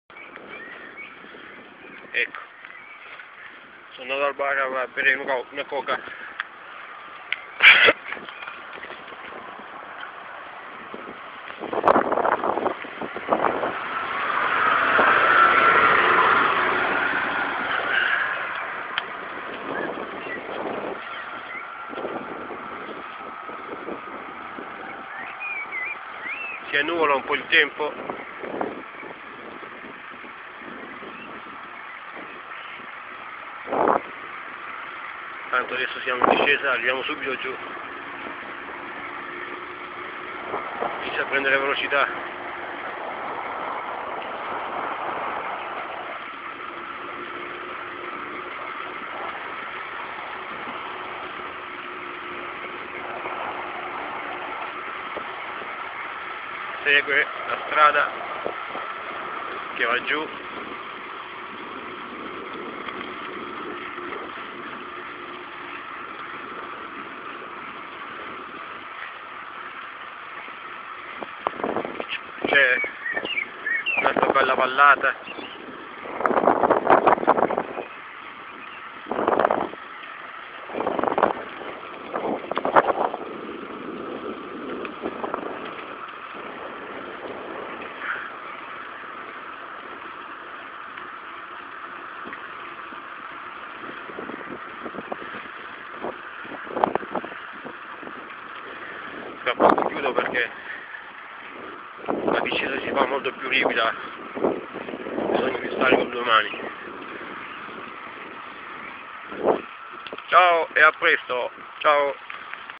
Discesa delle torricelle.mp4
inizio la discesa freni alla mano,e tornanti stretti via
giù a capo fitto.